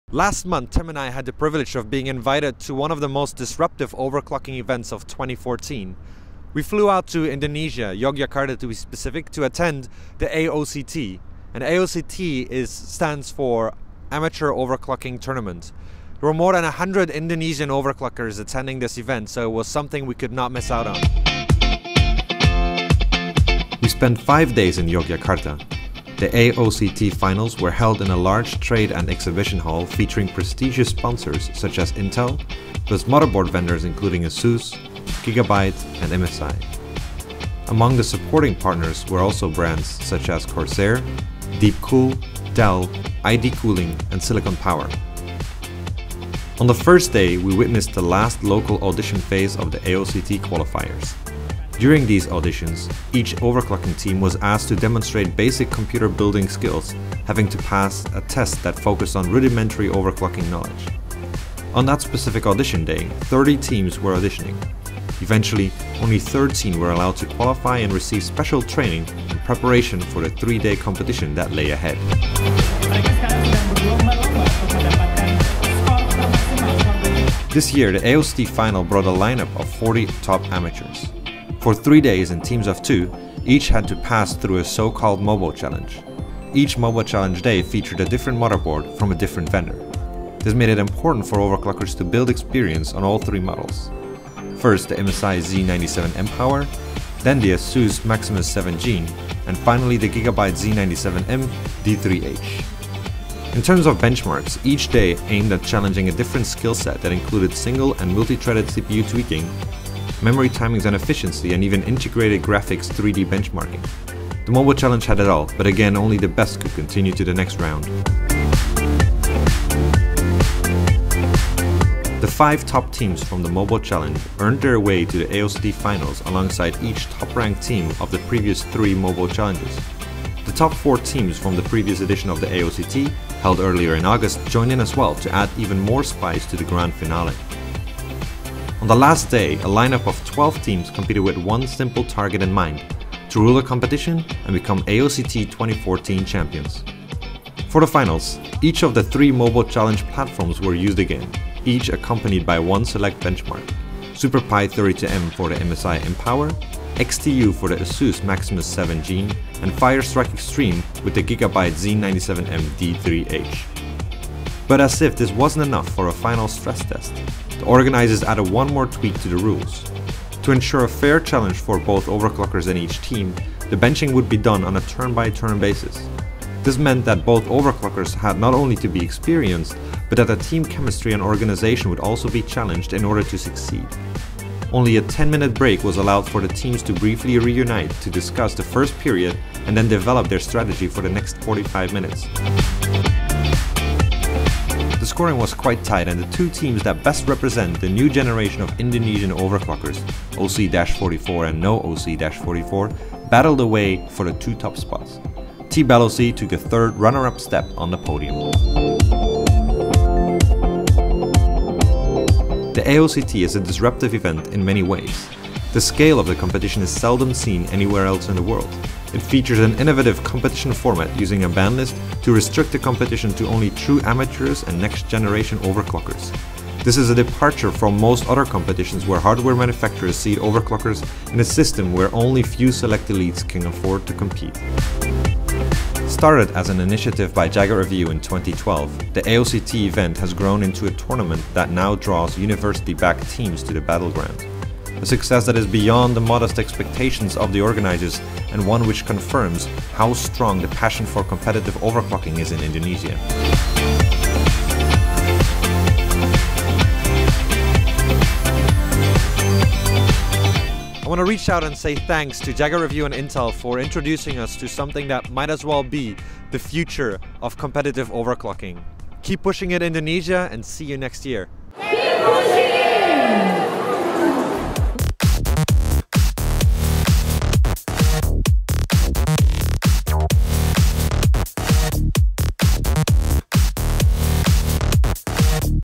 AOCT 2014 Official Aftermovie
This is the official aftermovie from Jagatreview.com 's Amateur Overclocking Tournament (AOCT) 2014 - The Final Battle.

More information and pictures of  this event can be found here:
Song :
Name of the album :
Download the complete song and Album for free from here :